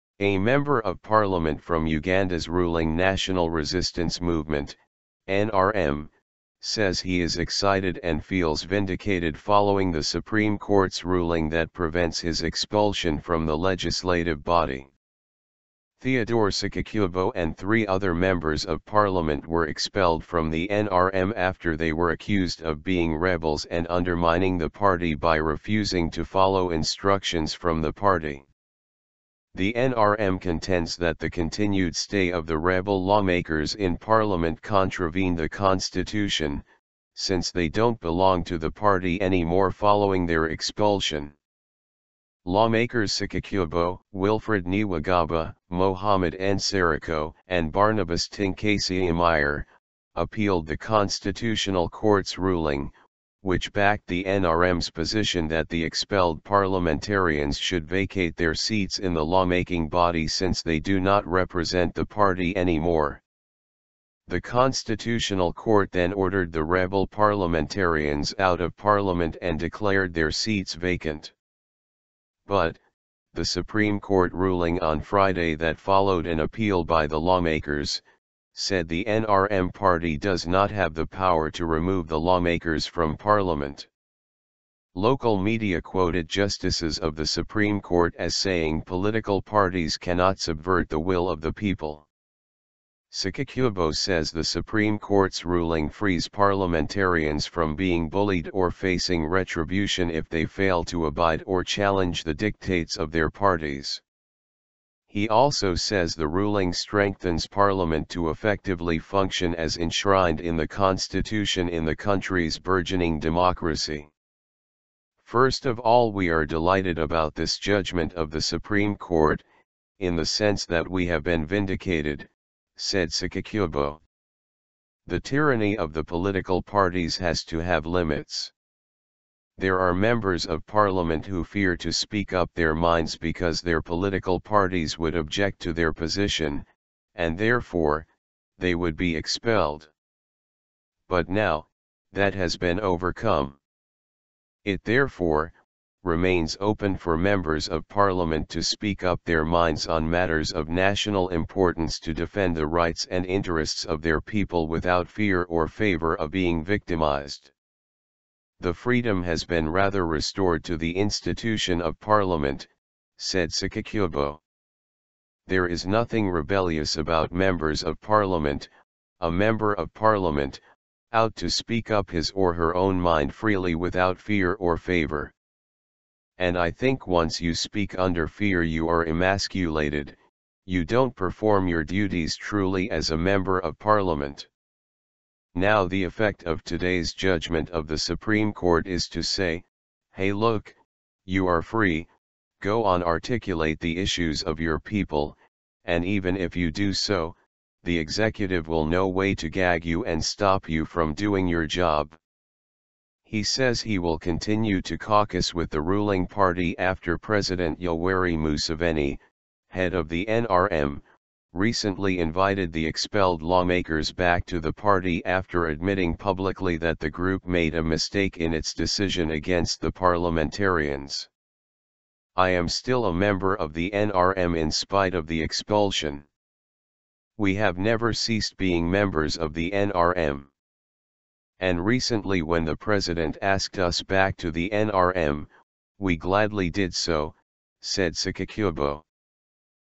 Uganda Supreme Court Rules in Favor of ‘Rebel Lawmakers’
A member of parliament from Uganda’s ruling National Resistance Movement (NRM) says he is excited and feels vindicated following the Supreme Court’s ruling that prevents his expulsion from the legislative body.

Theodore Ssekikubo and three other members of parliament were expelled from the NRM after they were accused of being rebels and undermining the party by refusing to follow instructions from the party. The NRM contends that the continued stay of the “rebel lawmakers” in parliament” contravened the constitution, since they don’t belong to the party anymore following their expulsion.

Lawmakers Ssekikubo, Wilfred Niwagaba, Mohammed Nsereko, and Barnabas Tinkasiimire, appealed the constitutional court’s ruling, which backed the NRM’s position that the expelled parliamentarians should vacate their seats in the lawmaking body since they do not represent the party anymore.

The Constitutional Court then ordered the “rebel parliamentarians” out of parliament and declared their seats vacant.

But, the Supreme Court ruling on Friday that followed an appeal by the lawmakers, said the NRM party does not have the power to remove the lawmakers from parliament. Local media quoted justices of the Supreme Court as saying “Political parties cannot subvert the will of the people.”

Ssekikubo says the Supreme Court’s ruling frees parliamentarians from being bullied or facing retribution if they fail to abide or challenge the dictates of their parties. He also says the ruling strengthens parliament to effectively function as enshrined in the constitution in the country’s burgeoning democracy.

“First of all we are delighted about this judgment of the Supreme Court, in the sense that we have been vindicated,” said Ssekikubo.

“The tyranny of the political parties has to have limits. There are members of parliament who fear to speak up their minds because their political parties would object to their position, and therefore, they would be expelled. But now, that has been overcome. It therefore, remains open for members of parliament to speak up their minds on matters of national importance to defend the rights and interests of their people without fear or favor of being victimized.”

“The freedom has been rather restored to the institution of parliament,” said Ssekikubo.

“There is nothing rebellious about members of parliament, a member of parliament, out to speak up his or her own mind freely without fear or favor. And I think once you speak under fear you are emasculated, you don’t perform your duties truly as a member of parliament. Now the effect of today’s judgment of the Supreme Court is to say, hey look, you are free, go on articulate the issues of your people, and even if you do so, the executive will no way to gag you and stop you from doing your job.”

He says he will continue to caucus with the ruling party after President Yoweri Museveni, head of the NRM, recently invited the expelled lawmakers back to the party after admitting publicly that the group made a mistake in its decision against the parliamentarians.

“I am still a member of the NRM in spite of the expulsion. We have never ceased being members of the NRM. And recently when the president asked us back to the NRM, we gladly did so,” said Ssekikubo.